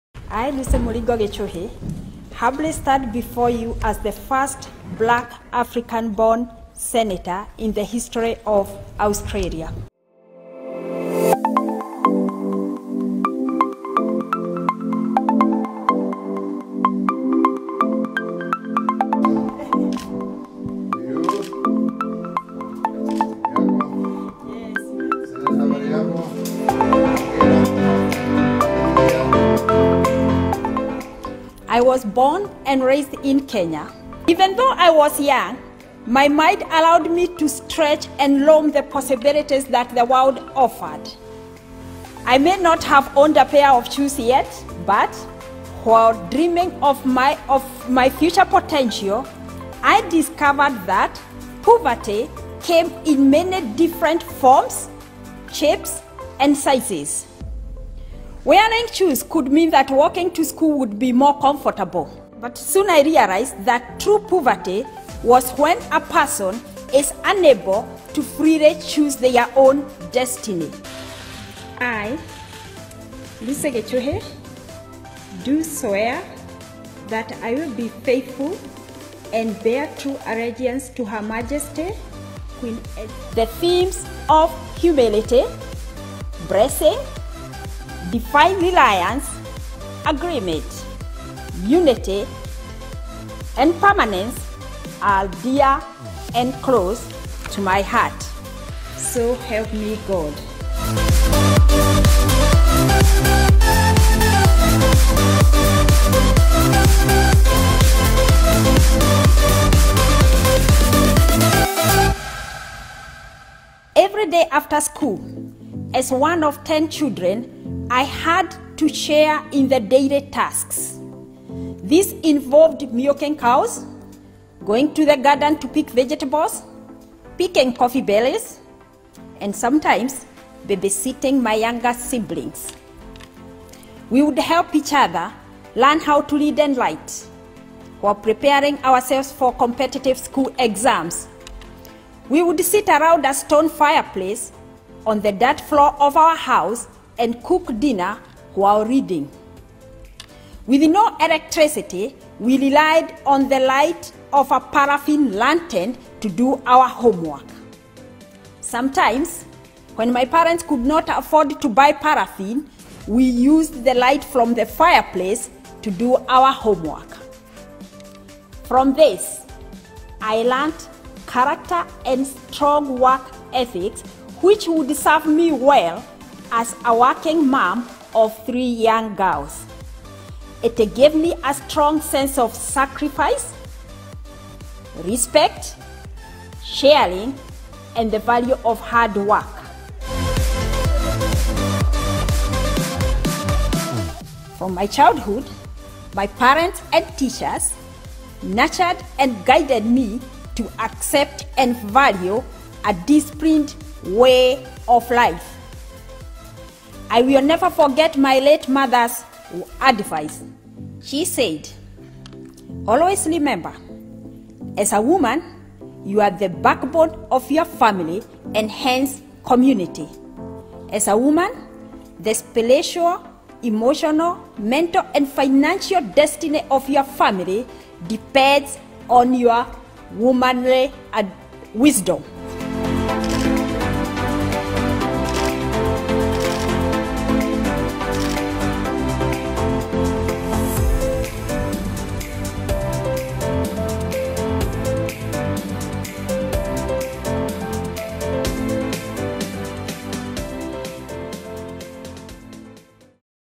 KENYAN BORN AUSTRALIAN SENATOR, LUCY GICHOHI
President Uhuru Kenyatta​ meeting the Kenyan born Australian Senator, Lucy Gichohi at State House.